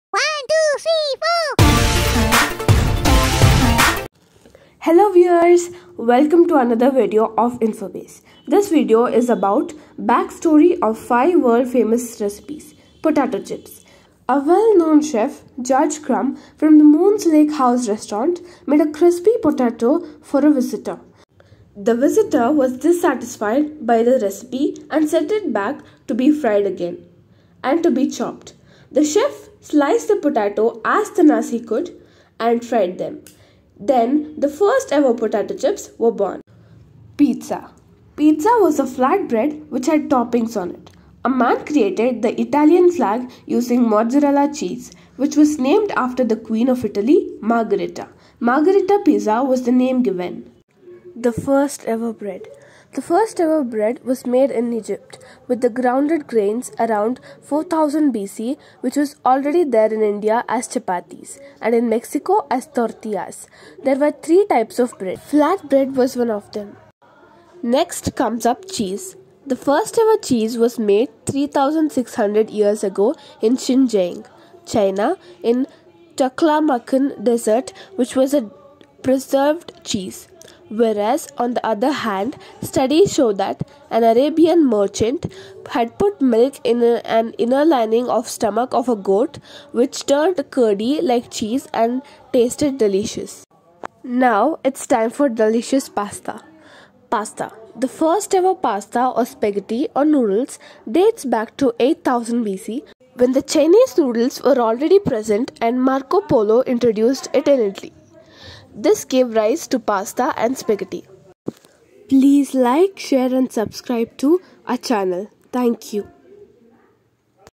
Backstory of 5 world famous recipes
Discover the story of introduction of world famous recipes. Find the interesting stories behind them.
1) Potato chips
2) Margherita Pizza
3) Bread
4) Cheese
5) Pasta and Noodles